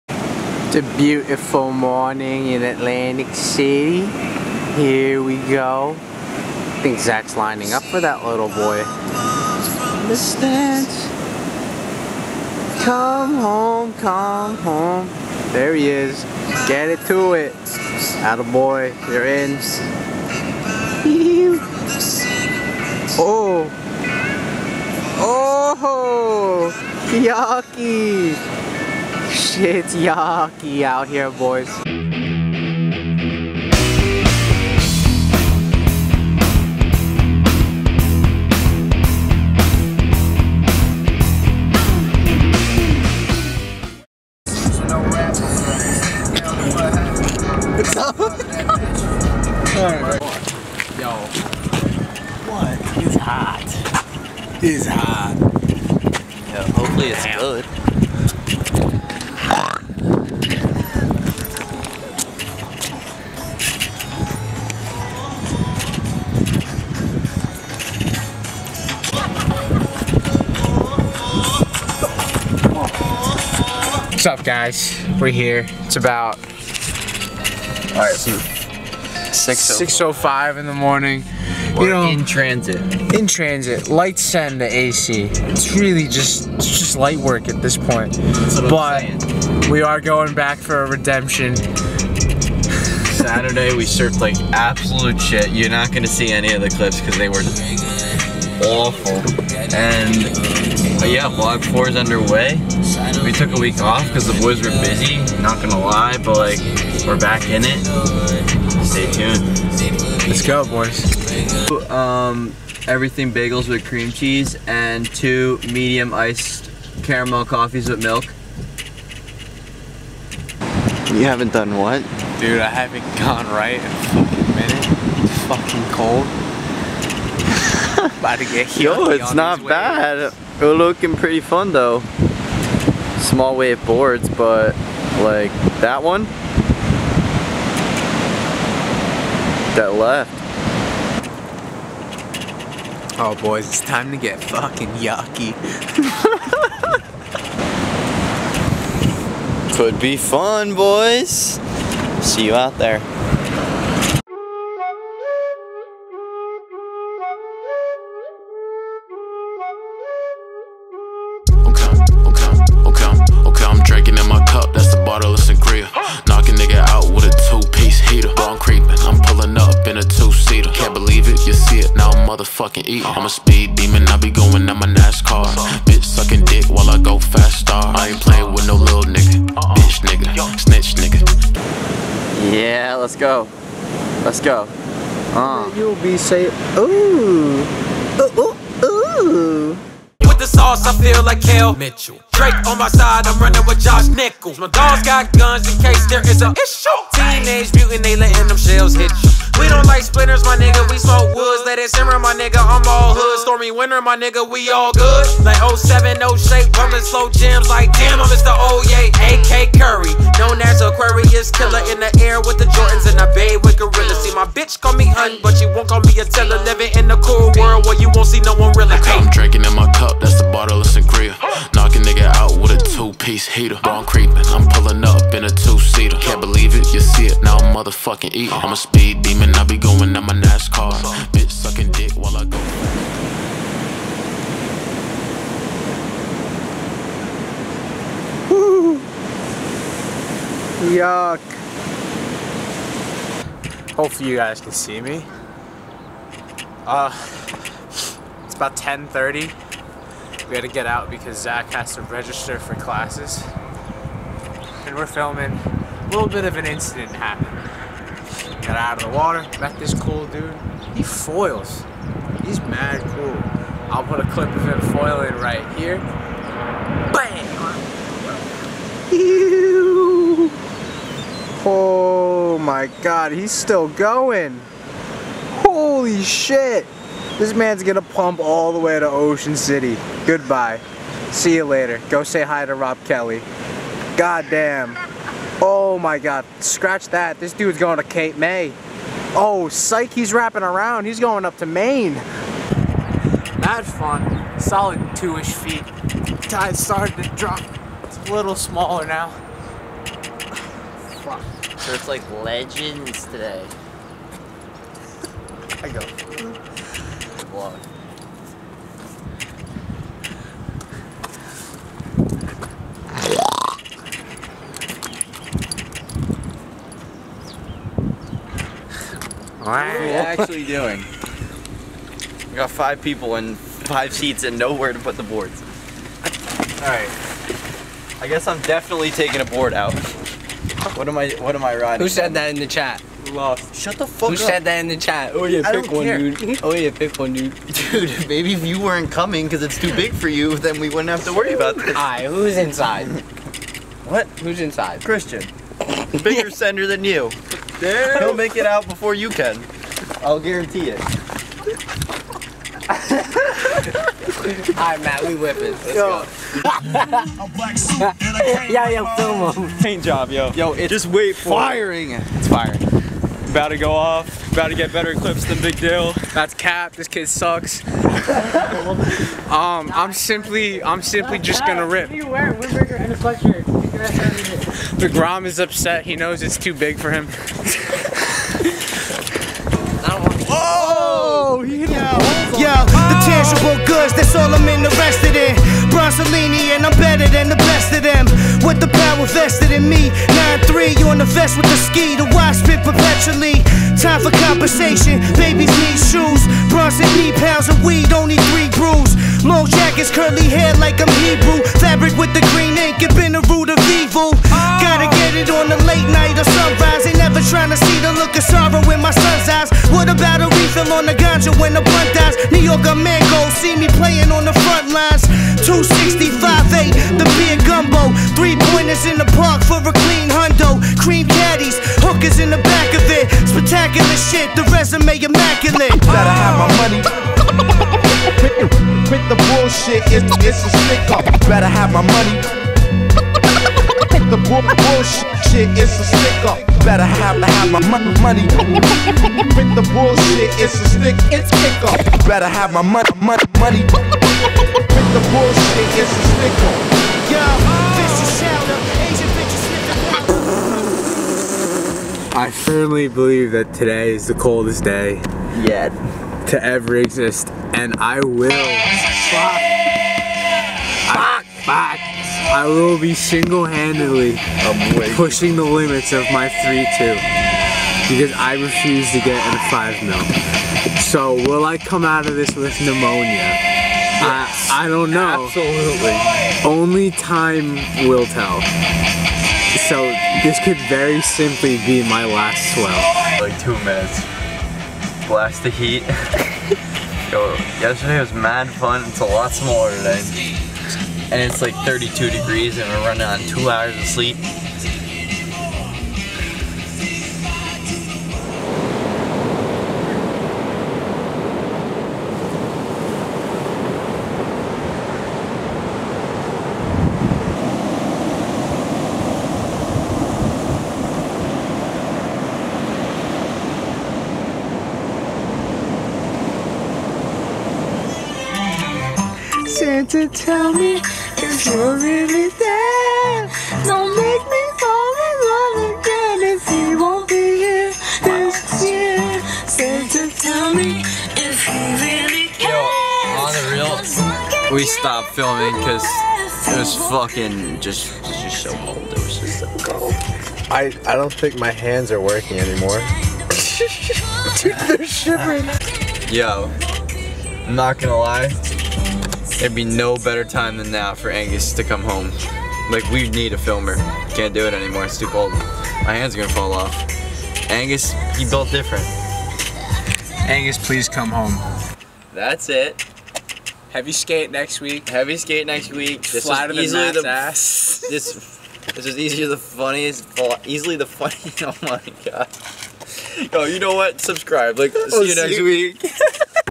Best week of surfing ever
In this week's vlog, enjoy the boys scoring multiple fun sessions.  Matt finally got to come home for some fun waves and Jersey started showing some signs of Winter.